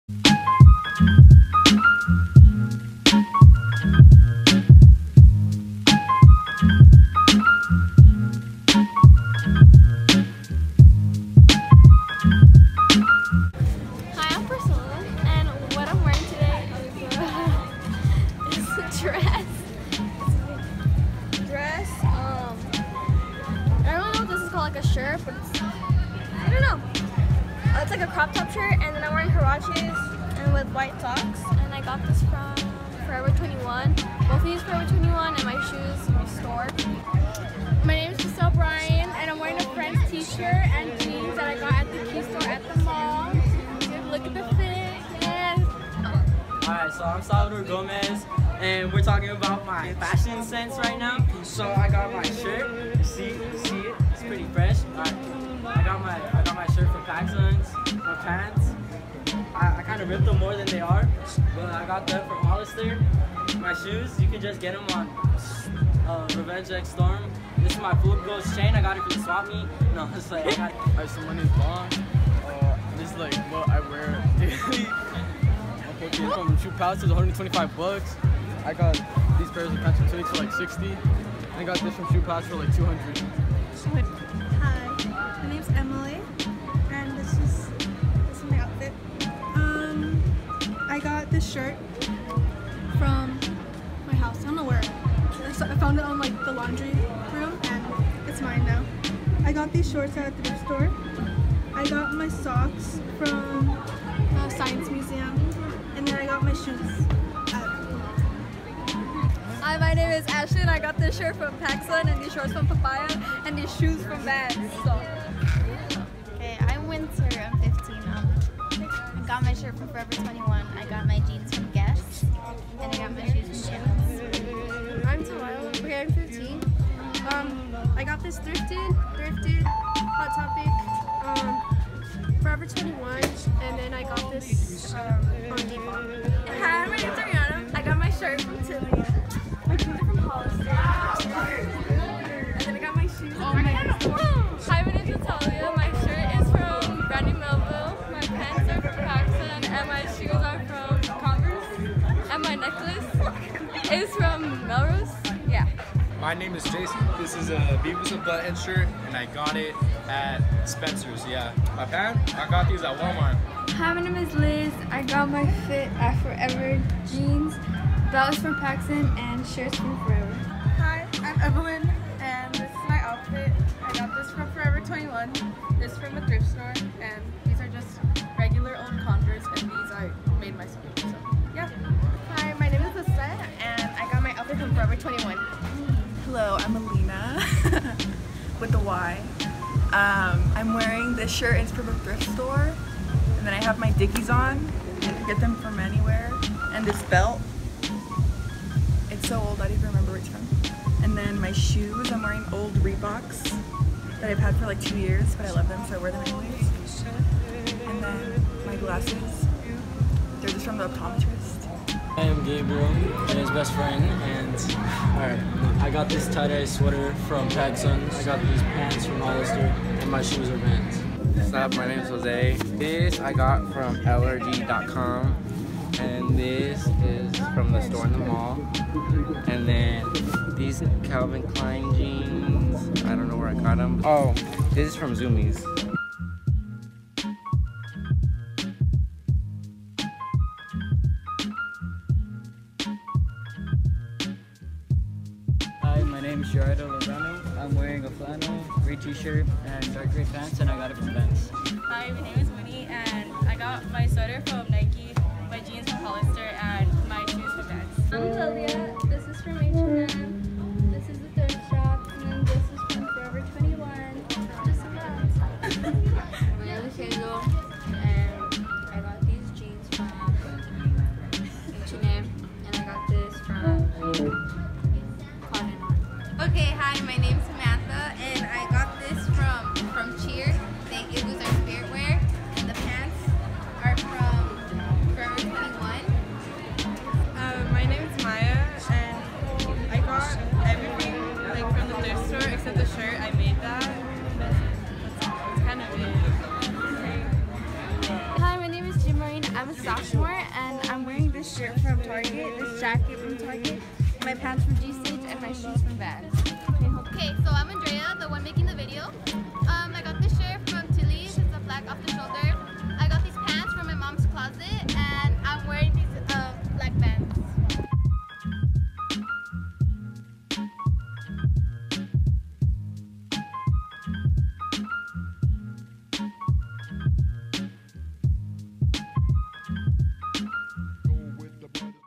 What high schoolers are ACTUALLY wearing | Dreuhhh
Here's what high schoolers are wearing in SoCal.
Thanks so much to everyone that let me record their outfits I couldn't have made this without you all.